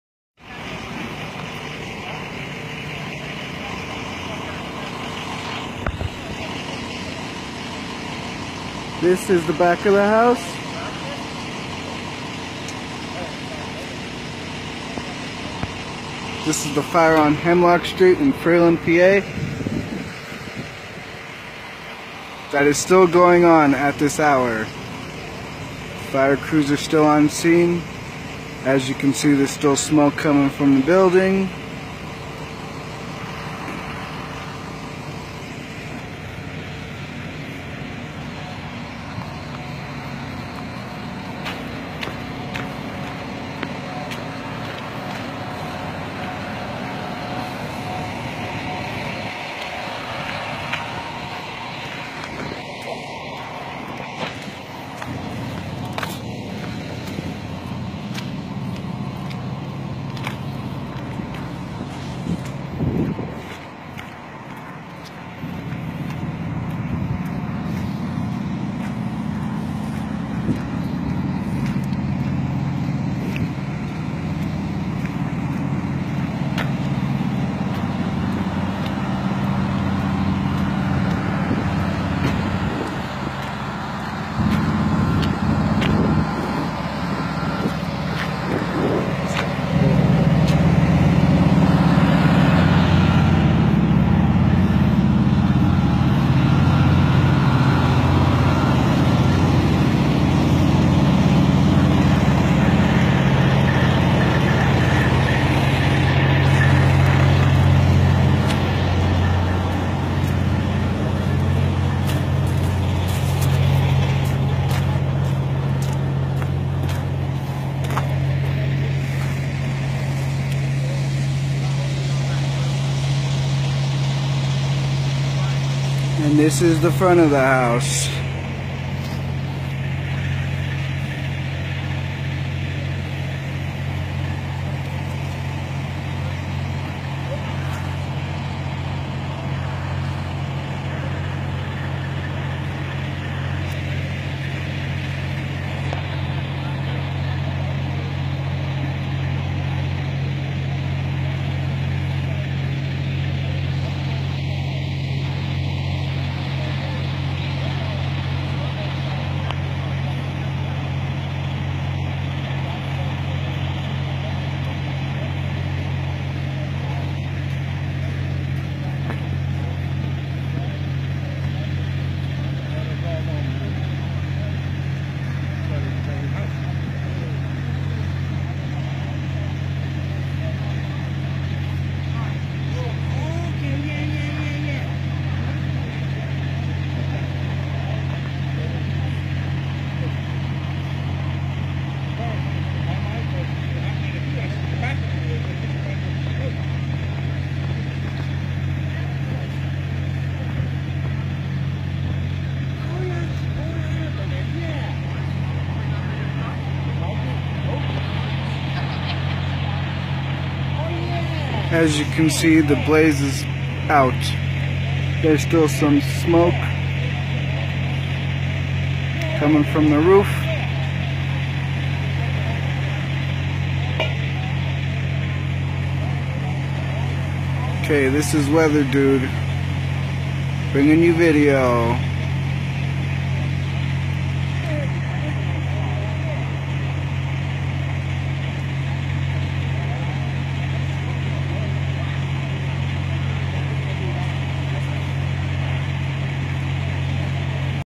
Hazleton on the rise news 8
Video taken about 20 minutes ago of the structure fire that is now out on Hemlock Street in Freeland PA most of the fire departments that were called out like West Hazleton and others have returned back to the station but a few still remain on scene there is still smoke coming from the roof as seen in the video

Posted by weather dude